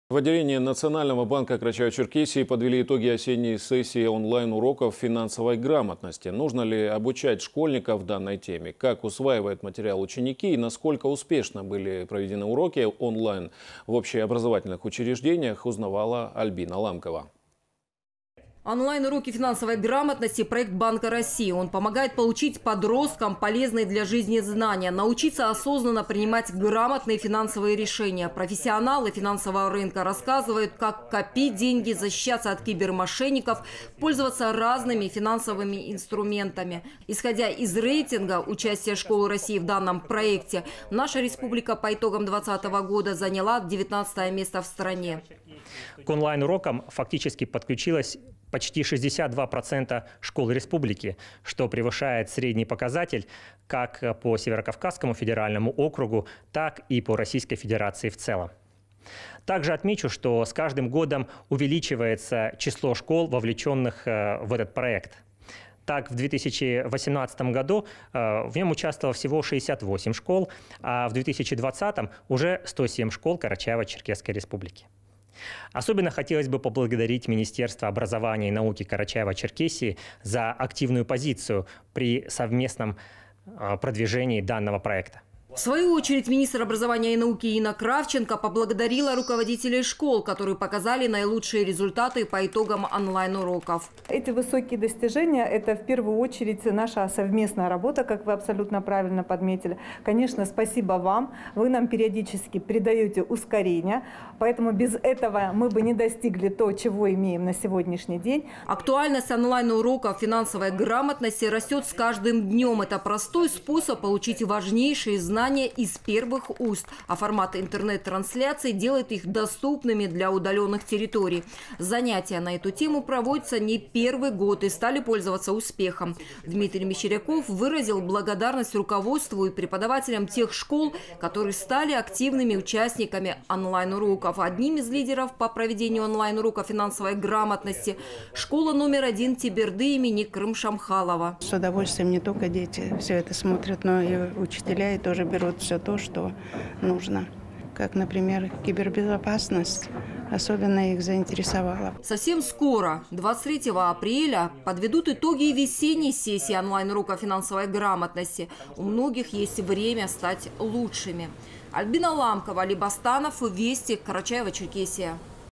В отделении Национального банка подвели итоги осенней сессии онлайн-уроков финансовой грамотности
В отделении Национального банка Карачаево-Черкесии подвели итоги осенней сессии онлайн-уроков финансовой грамотности